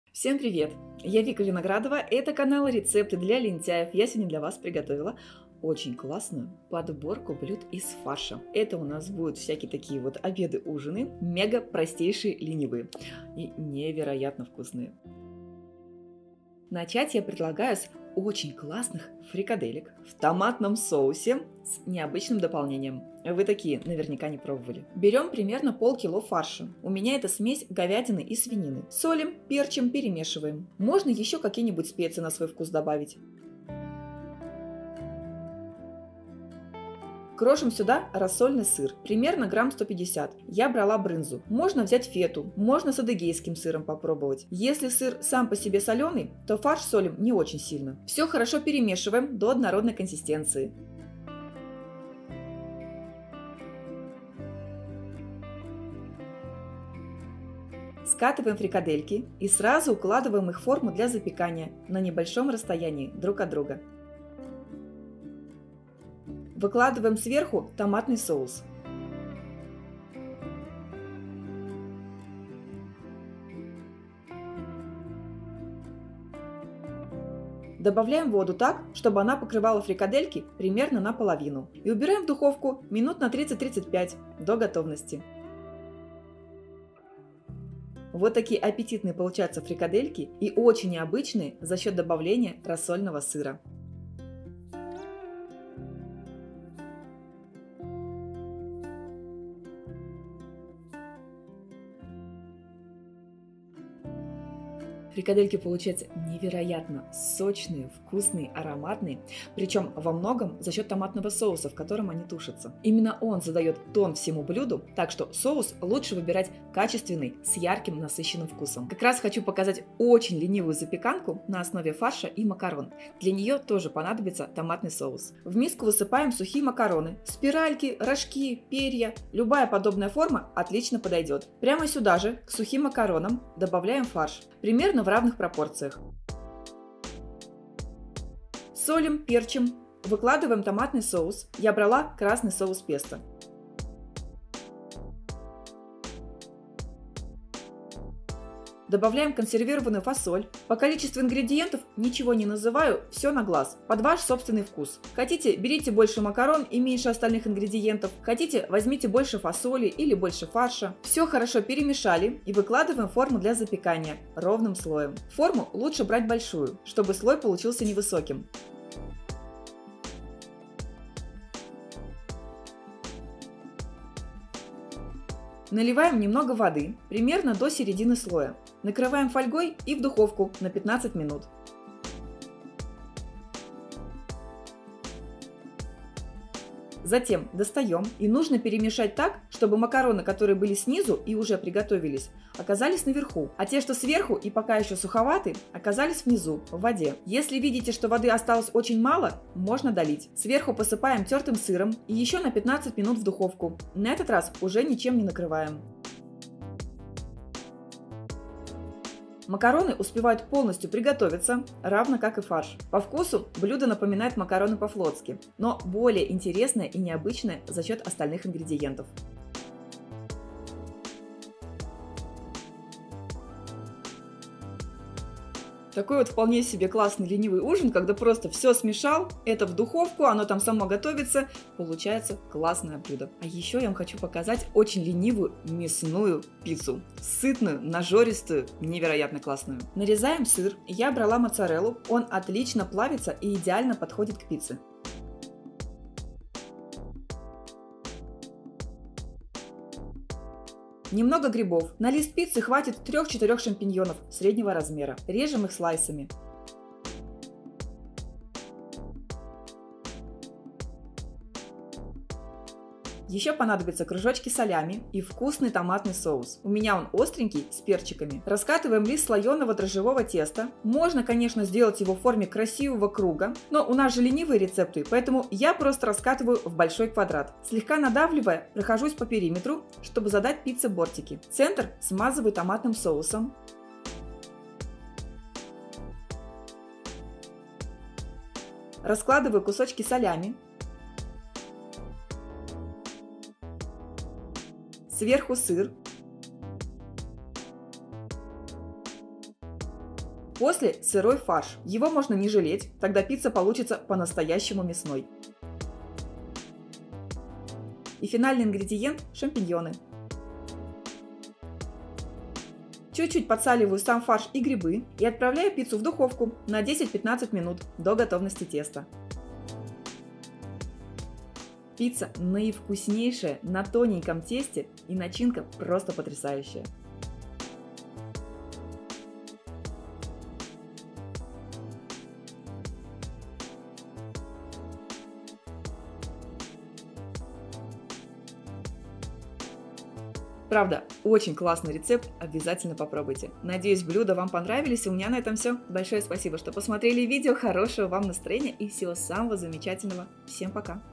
БЫСТРЫЕ и ВКУСНЕЙШИЕ Рецепты из ФАРША на Каждый День! Такие Рецепты Понравятся ВСЕМ!
✳️ФРИКАДЕЛЬКИ В ТОМАТНОМ СОУСЕ

Ингредиенты:
- 500гр фарша
- 150гр брынзы
- Томатный соус

Фарш солим и перчим, добавляем сыр и перемешиваем до однородной массы. Формируем фрикадельки и выкладываем в форму. Сверху выкладываем томатный соус, добавляем воду и запекаем в духовке 30-35 минут при 180 градусах.

✳️ ЗАПЕКАНКА ИЗ ФАРША И МАКАРОН

Ингредиенты:
- Макароны
- Фарш
- Томатный соус
- Консервированная фасоль
- Сыр

В сухие макароны добавляем фарш, солим, перчим, добавляем томатный соус и фасоль. Перемешиваем и выкладываем в форму для запекания. Заливаем водой, накрываем фольгой и отправляем в духовку при 180 градусах на 15 минут.
Достаем, перемешиваем, добавляем воды, посыпаем сыром и снова в духовку на 15 минут при 180 градусах.

✳️ МЯСНАЯ ПИЦЦА

Ингредиенты:
- Сыр
- Грибы
- Салями
- Томатный соус
- Слоеное дрожжевое тесто
- Фарш

Нарезаем сыр и грибы. Тесто раскатываем, смазываем соусом, выкладываем салями, сыр, грибы и фарш. Солим и отправляем в духовку на 10-15 минут при 180 градусах.